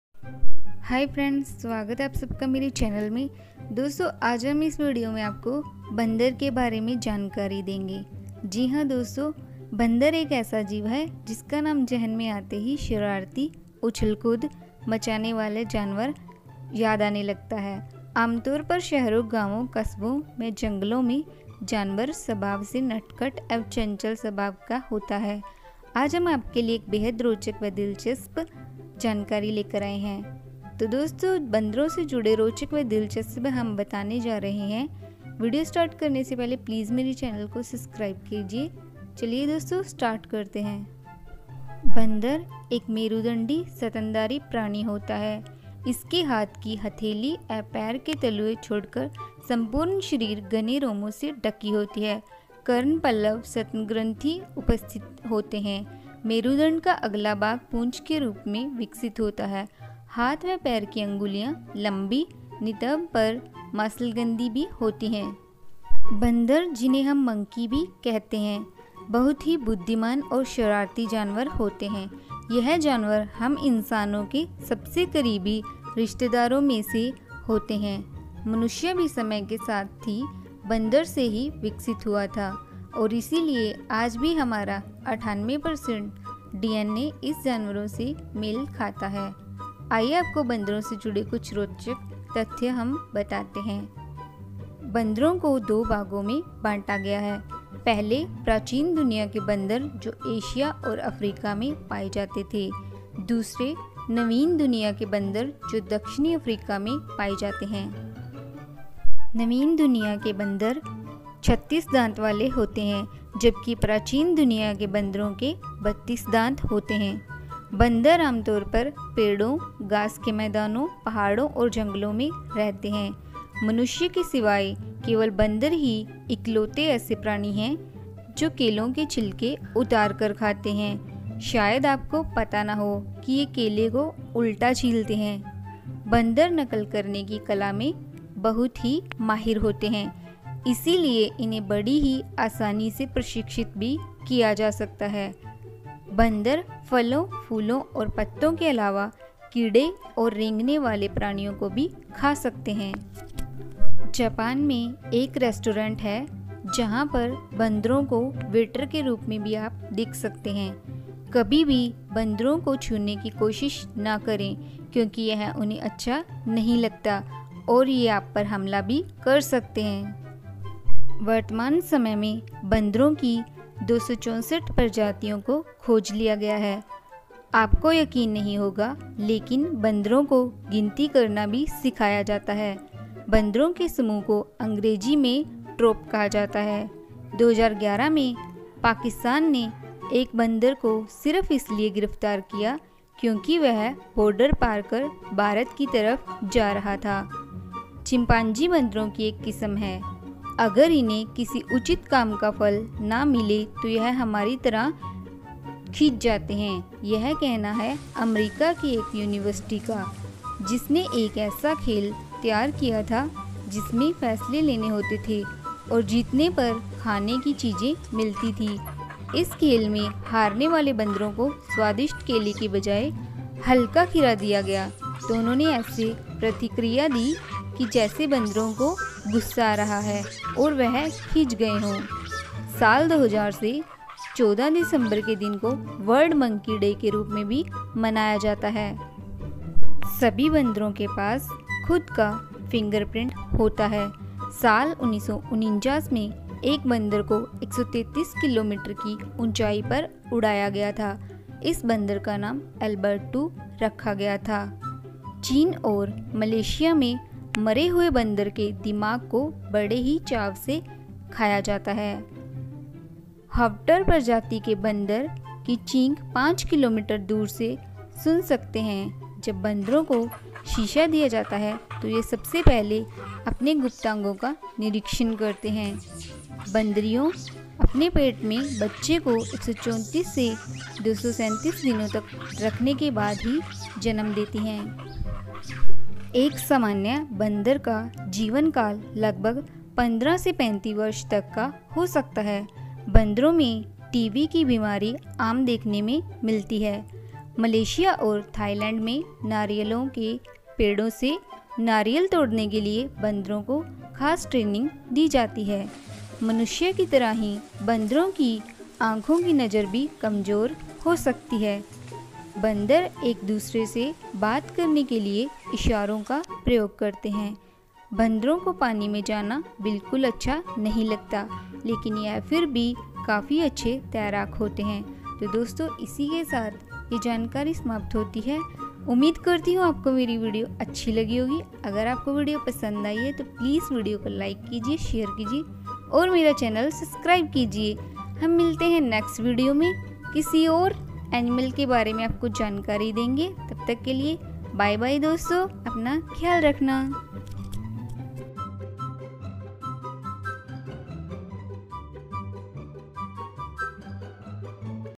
Monkey Facts in Hindi - बंदरों के बारे में जानकारी | Trending Topics
Dosto is video mein humne aapko monkey ke bare mein full jankari di h plz video ko end tk jrur dekhe.